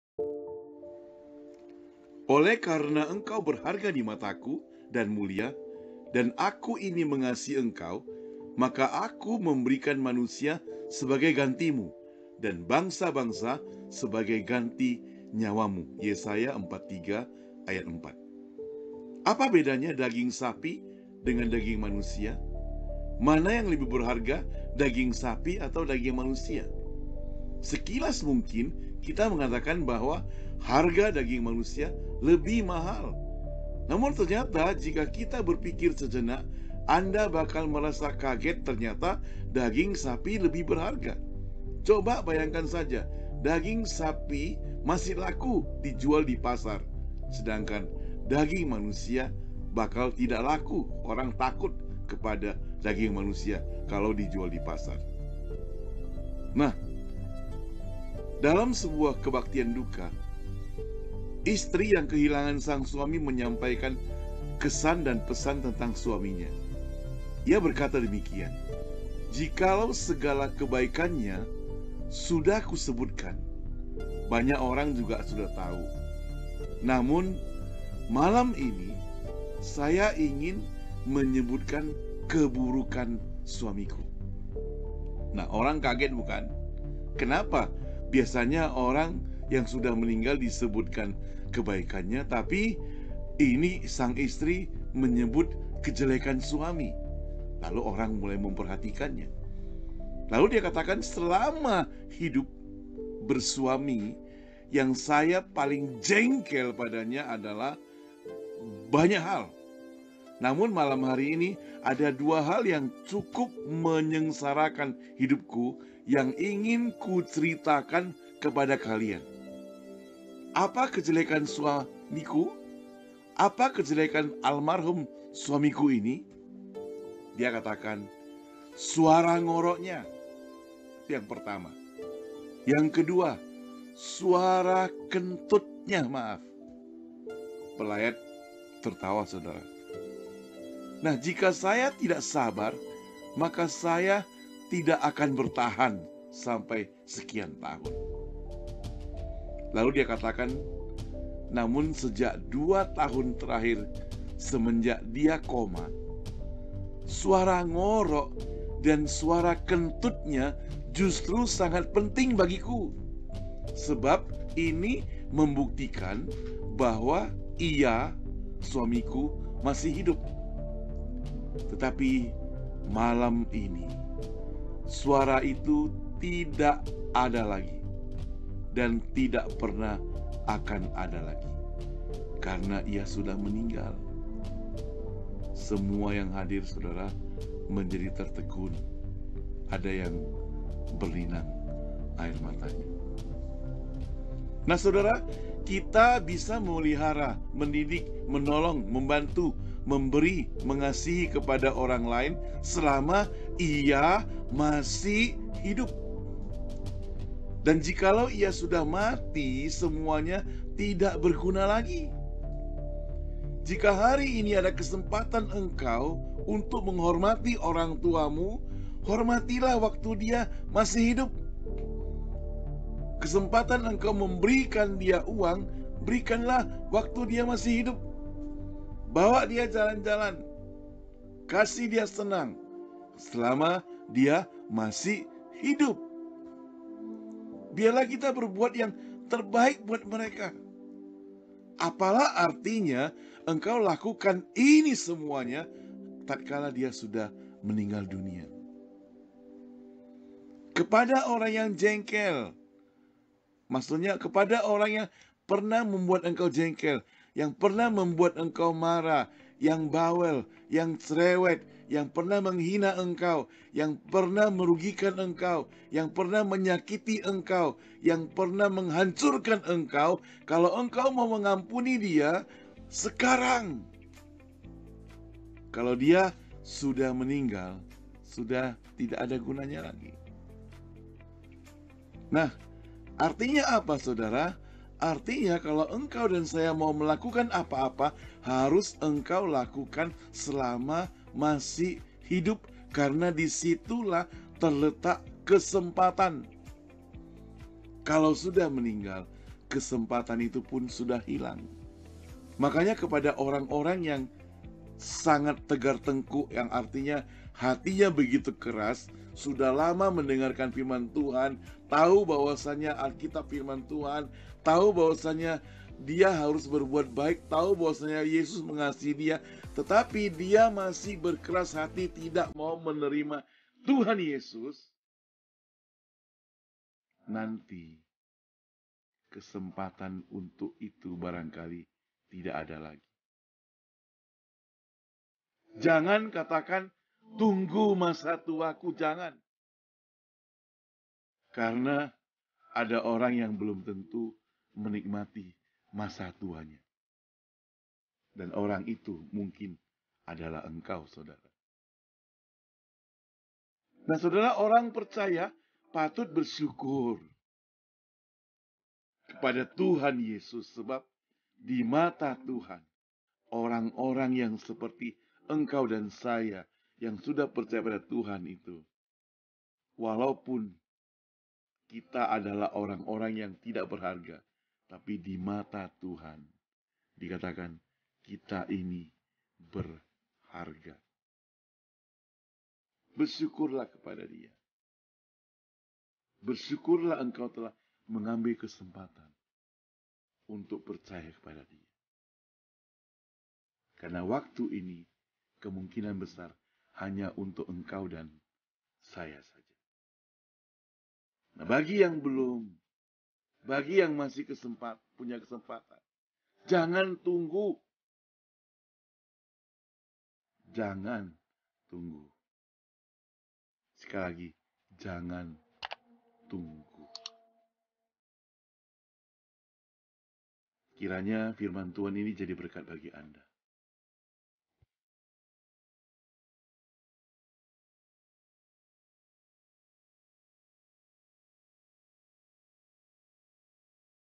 MANA LEBIH BERHAGA? DAGING SAPI ATAU DAGING MANUSIA??
MANA LEBIH BERHARGA, DAGING SAPI ATAU DAGING MANUSIA?
                Oleh karena engkau berharga di mata-Ku dan mulia, dan Aku ini mengasihi engkau, maka Aku memberikan manusia sebagai gantimu, dan bangsa-bangsa sebagai ganti nyawamu. (Yesaya 43:4)
  Apa bedanya daging sapi dengan daging manusia? Mana yang lebih berharga daging sapi atau daging manusia? Sekilas mungkin kita menngatakan bahwa harga daging manusia lebih mahal, namun ternyata jika kita berpikir sejenak, anda bakal merasa kaget ternyata daging sapi lebih berharga. Coba bayangkan saja, daging sapi masih laku dijual di pasar dan harganya memadai sedangkan daging manusia tidak laku dijual bahkan gratis diberikan kepada orang lainpun mereka tidak mau menerimanya.
   Dalam sebuah Kebaktian Duka , istri yang kehilangan sang suami menyampaikan kesan dan pesan tentang suaminya. Ia berkata demikian, jikalau segala kebaikannya sudah disebutkan banyak orang, namun malam ini saya ingin menyebutkan keburukannya. Semua yang hadir kaget, apa gerangan?. Lalu dia katakana,selama hidup bersuami yang saya paling jengkel padanya ada banyak hal, namun ada dua hal yang cukup menyengsarakan, yakni suara ngorok dan kentutnya (Pelayat tertawa)  Jika saya tidak sabar, maka saya tidak akan bertahan. Lalu dia katakan namun sejak dua tahun terakhir semenjak dia koma, suara ngorok dan kentut justru sangat penting bagiku, sebab ini membuktikan ia masih hidup. Tetapi malam ini, suara itu tidak ada dan tidak pernah ada lagi, karena ia sudah meninggal. Semua yang hadir menjadi tertegun diam.
Kita bisa memelihara, mendidik, menolong, membantu, memberi, mengasihi kepda orang lain selama ia masih hidup, jakalau ia sudah mati semuanya tidak berguna lagi.  Jika hari ini ada kesempatan biarlah kita berbuat sesuatu buat mereka yang masih hidup, mungkin orang itu pernah menjengkeli anda, buat anda marah, bawel, cerewet ;  itu pertanda dia masih ada bersama-sama anda. Seorang manusia apapun jabatannya, seberapa besarpun kekayaannya, jika sudah mati maka tidak ada gunanya lagi. Orang percaya patut bersyukur, sebab di mata Tuhan orang-orang yang mengashiNya berharga dimataNya.
Terima kasih Bapa, Engkau yang penuh kasih. Ajarilah aku supaya kehidupanku ini boleh bermakna bagi ornag-orang sekitar aku. Aku mohon ampun jika selama hidup ini telah mebuat orang lain susah dan merugikan mereka. Atas nama Tuhan Yesus aku berdoa, Amin. (SS)